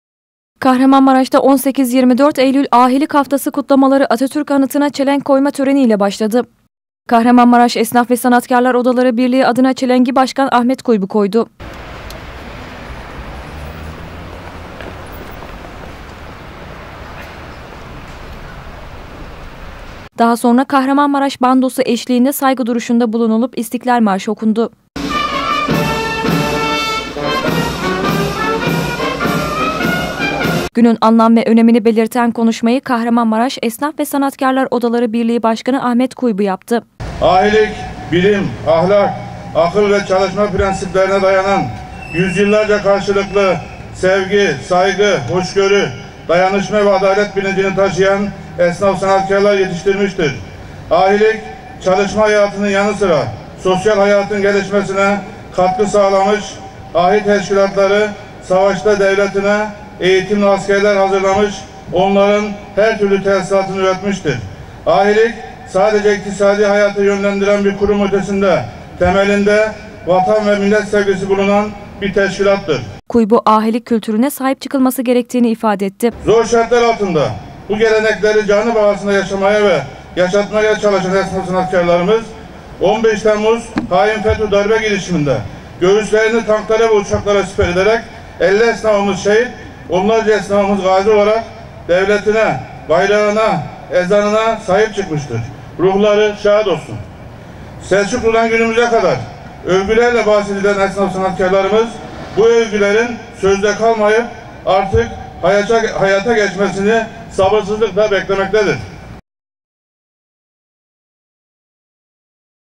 Ahilik Haftası Törenle Başladı
K.Maraş’ta Ahilik Haftası Nedeni İle Düzenlenen Etkinlikler Bugün Atatürk Anıtı Önüne Çelenk Sunumu İle Başladı.